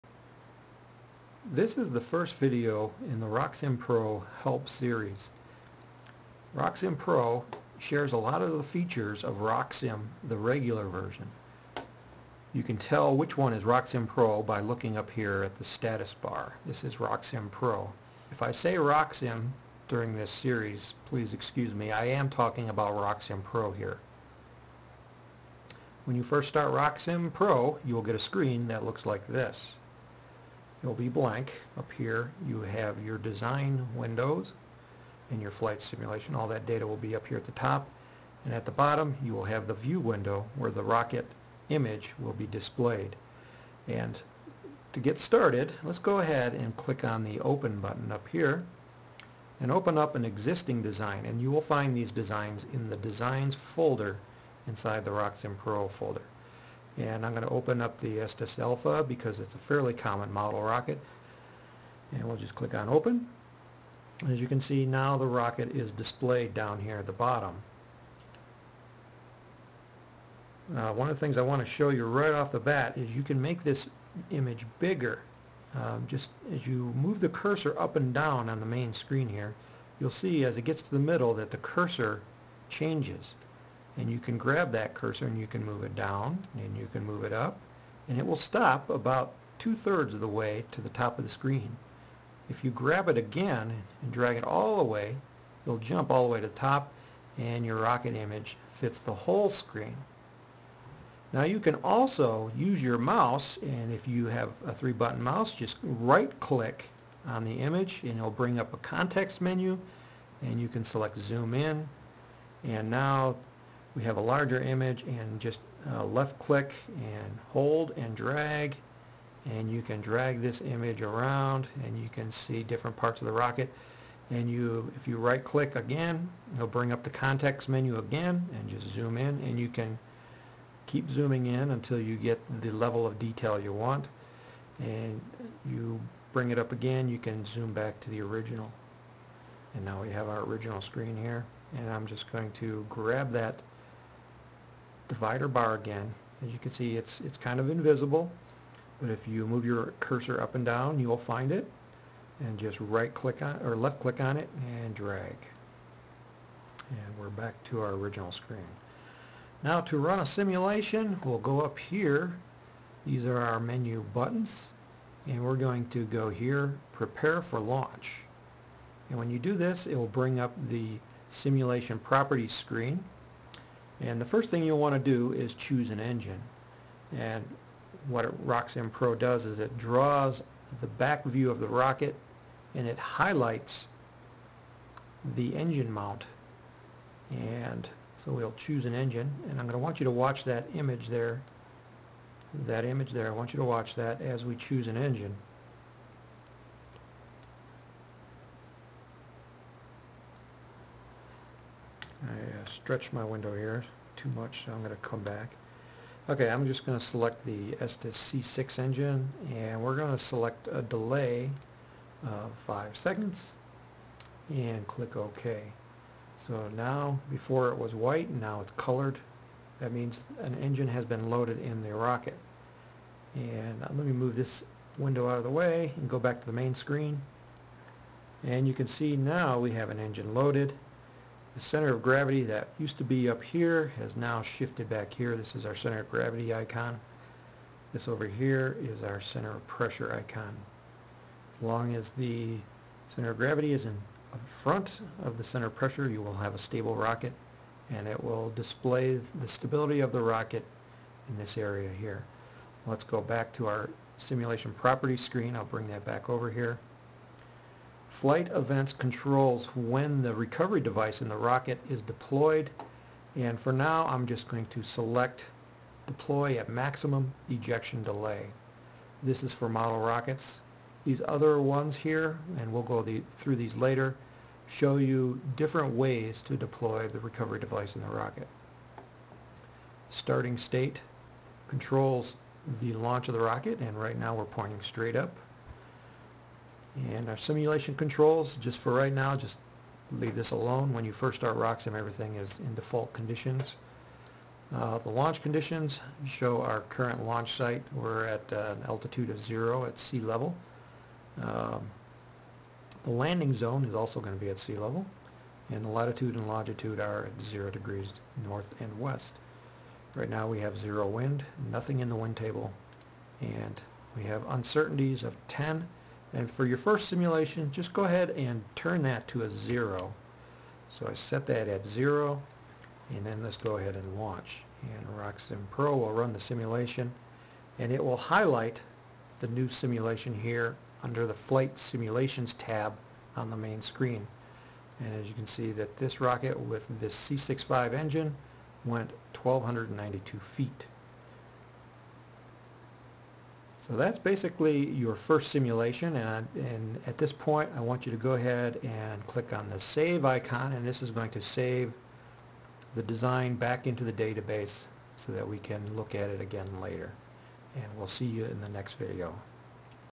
RockSim Pro Software Tutorials - 1. Getting Started
- Get Started by opening an existing design & run a simple simulation